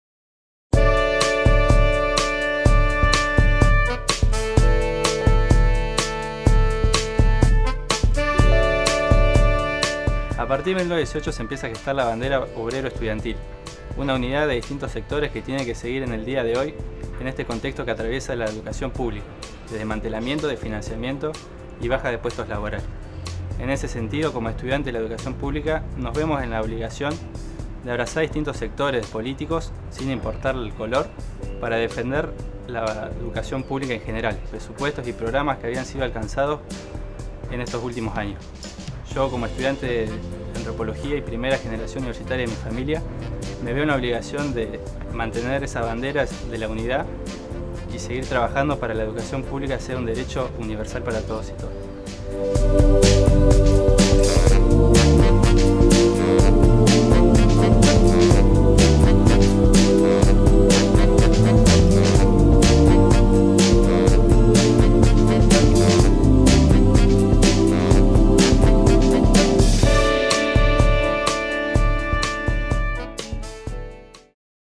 Gastón. A 100 años de la Reforma en la FFyH.
Los estudiantes de la FFyH hablan sobre los derechos conquistados en 1918 y los desafíos que plantea el siglo XXI.
Video realizado por el Área de Tecnología Educativa de la FFyH en conjunto con el Área de Comunicación Institucional de la Facultad.